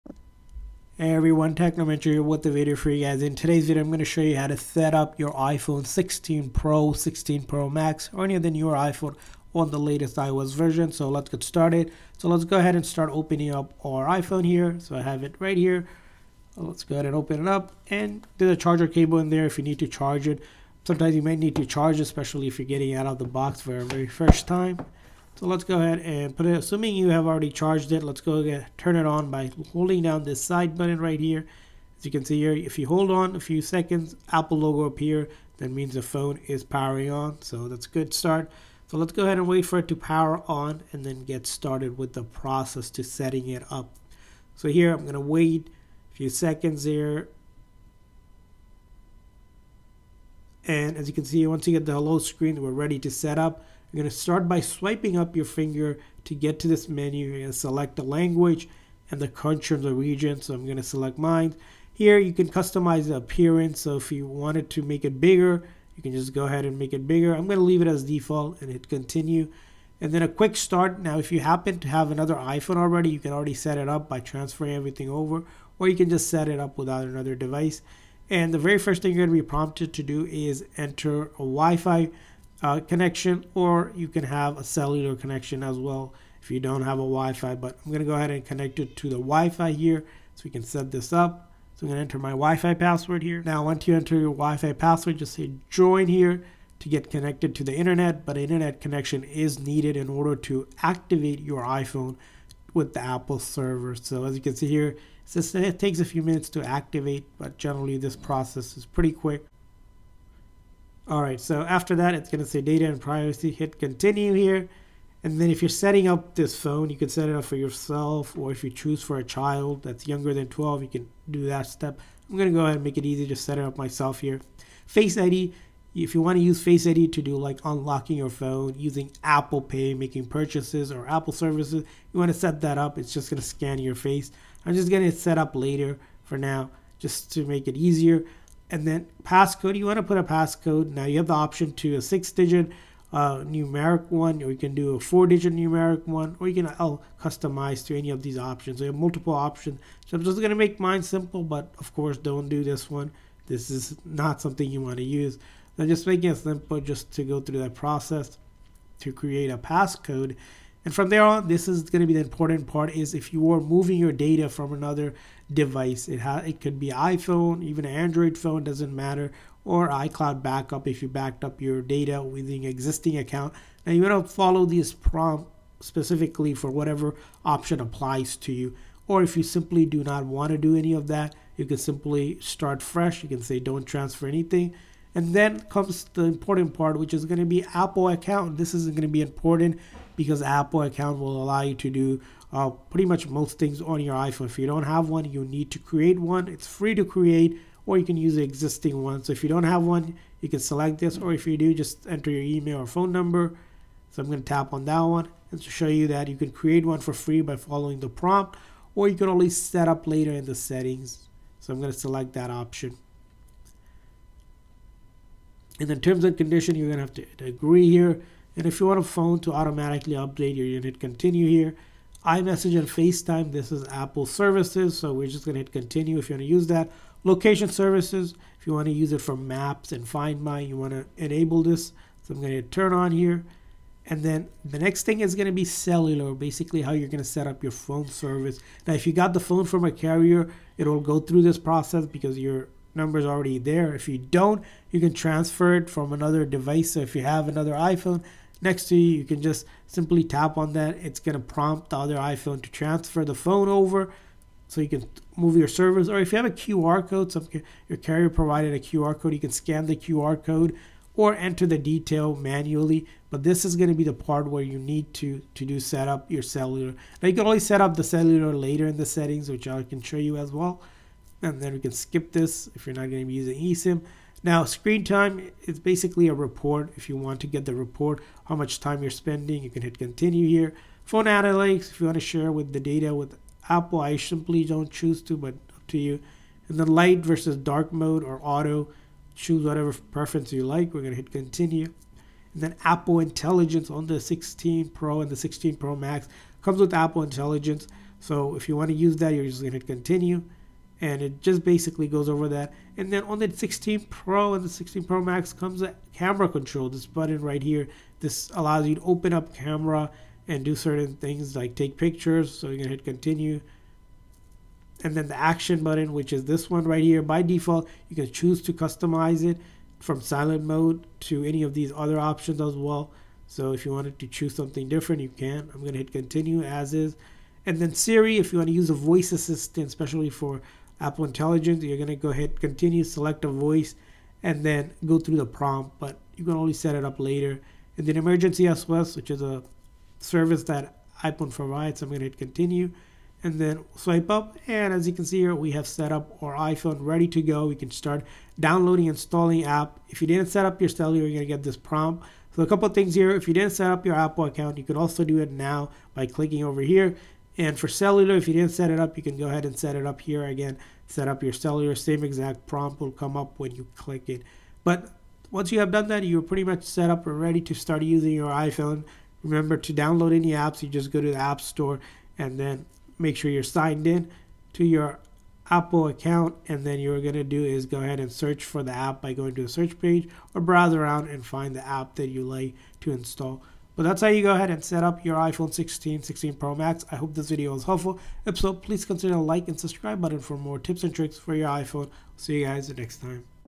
iPhone 16 Pro / Pro Max: How To Set Up (Step by Step)
In this tutorial, you will learn how to set up and activate iPhone 16 Pro / 16 Pro Max!